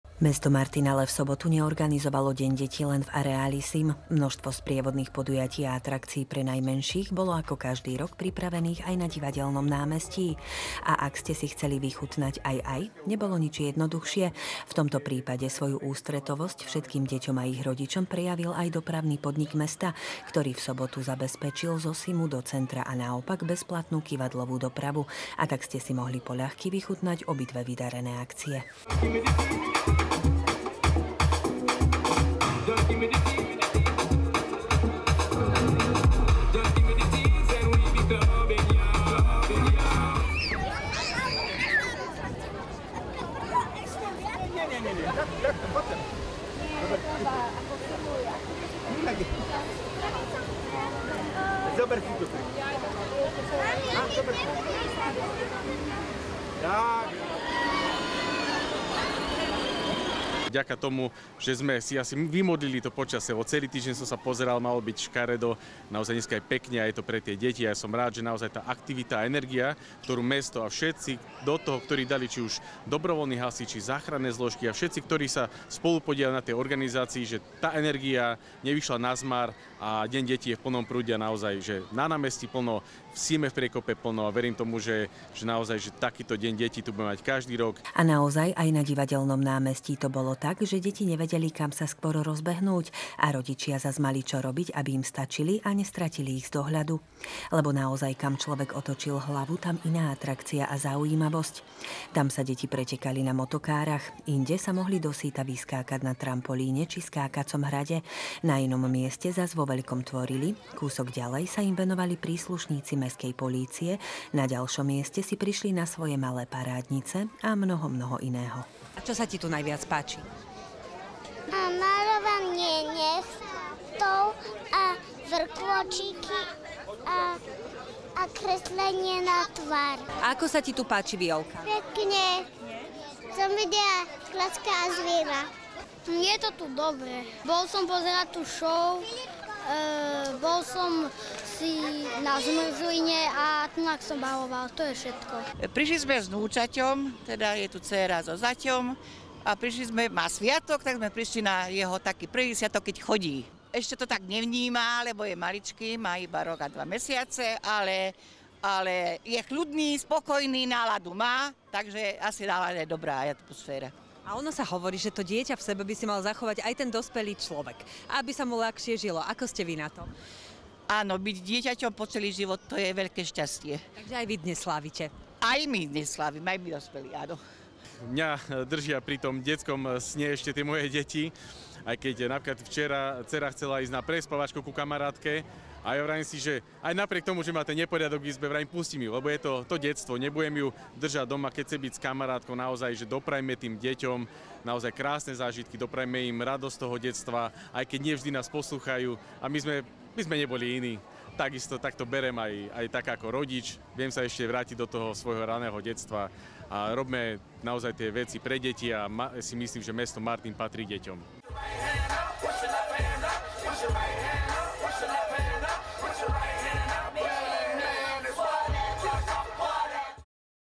Aj Divadelné námestie patrilo v sobotu deťom
V sobotu prebiehali v meste súbežne hneď dve veľké akcie určené deťom a ich medzinárodnému dňu. Aby sa deti spolu s rodičmi mohli pohodlne dostať z jedného miesta na druhé, v ústrety im vyšiel aj Dopravný podnik mesta Martin, ktorý v sobotu zabezpečil bezplatnú kyvadlovú dopravu. Na Divadelnom námestí bolo tiež množstvo atrakcií, deti sa mohlo povoziť na motokárach, vyskákať sa na trampolíne či skákacom hrade, pripravené boli tvorivé dielničky, malé parádnice si mohli nechať urobiť vrkôčiky či namaľovať tvár a nechty a mnoho mnoho iného.